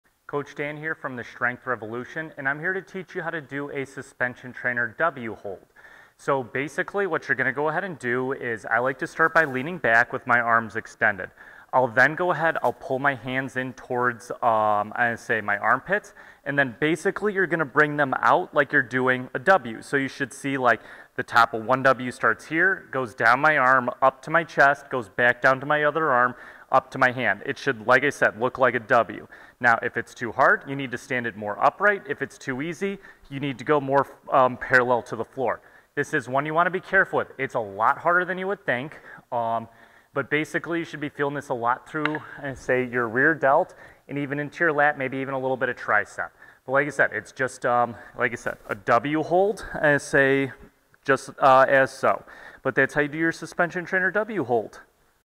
Suspension Trainer W Hold
Try out this back and shoulder static hold